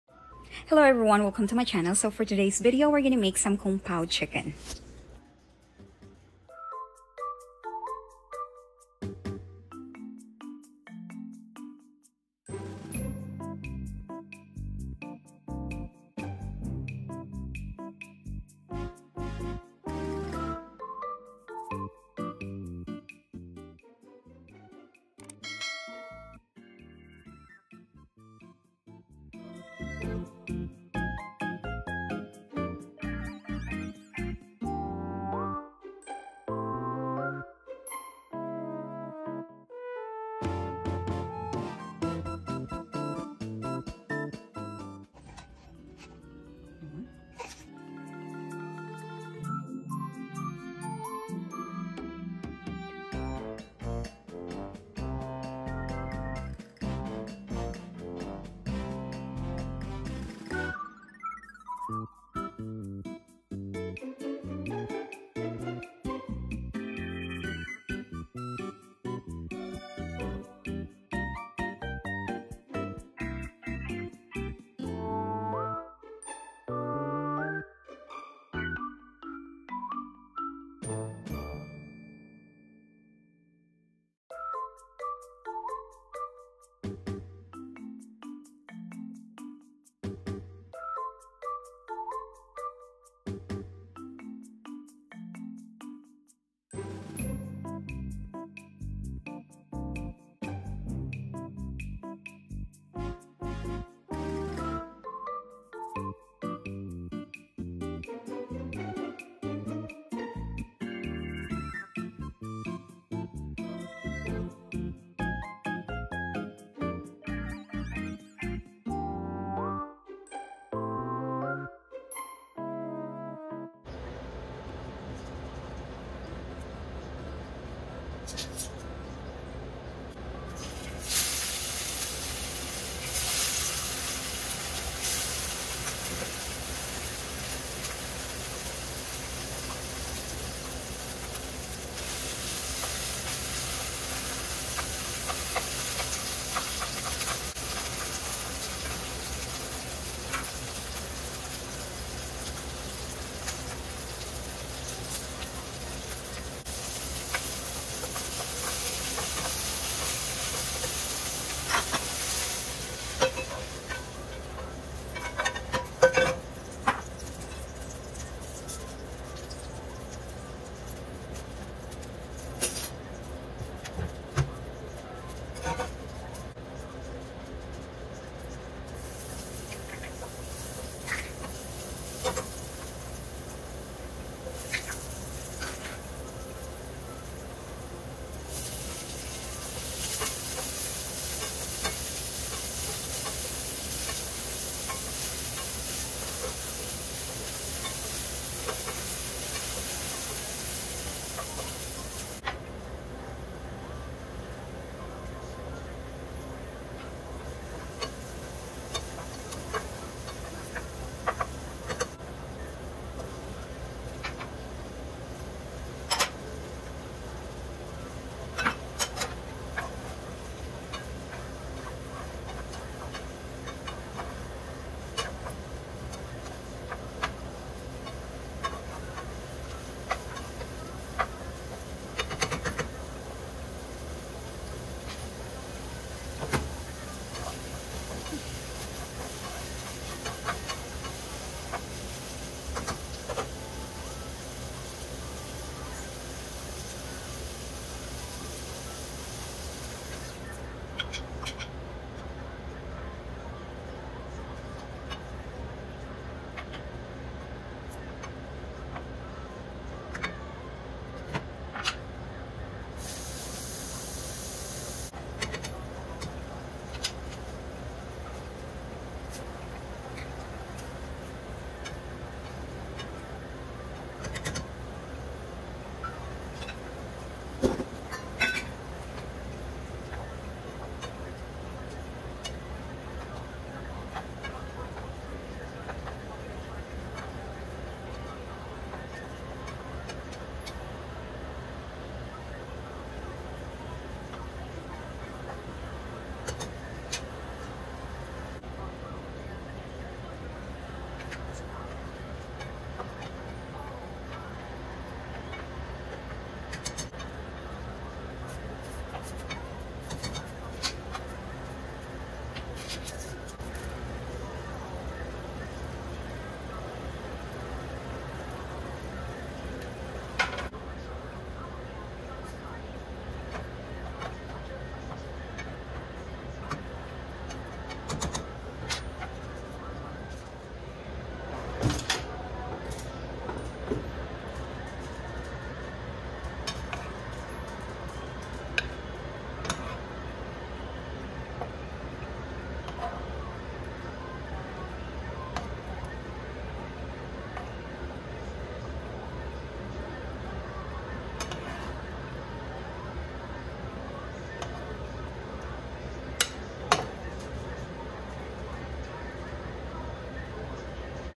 Delicious Kung PAO Chicken
HelloWelcome to my channel and thank you for turning up so for today’s video we’re gonna make some Kung PAO Chicken. I hope you guys like it and enjoy watching.￼